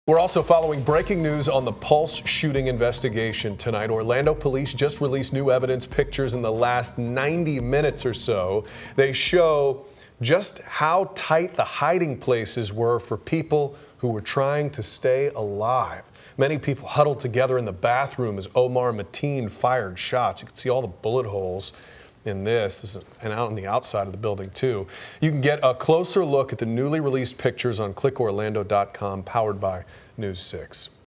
Police release new evidence photos from Pulse
New evidence photos from inside Pulse nightclub were released Tuesday afternoon.